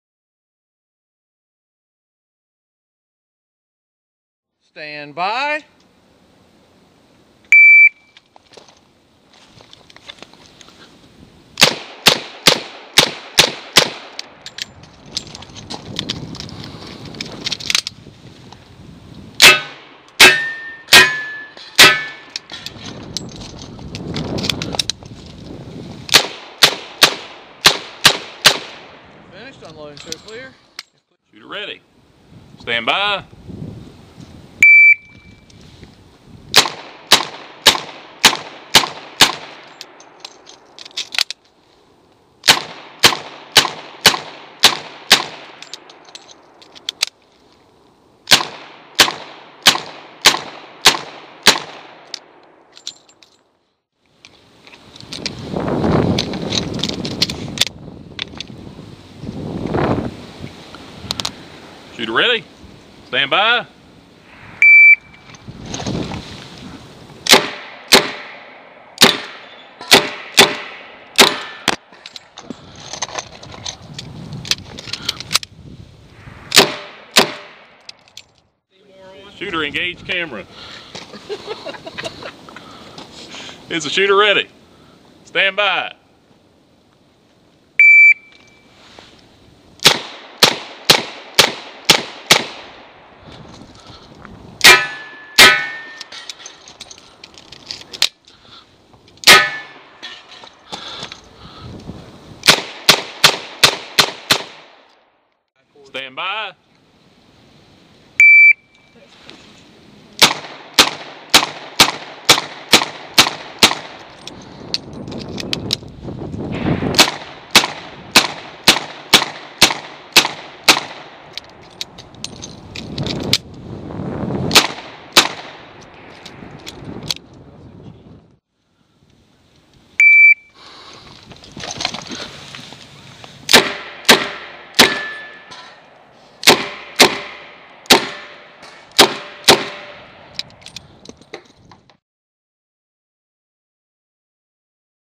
ICORE match 2-13-11 Hat Cam
ICORE match at putnam county gun club first try at hat cam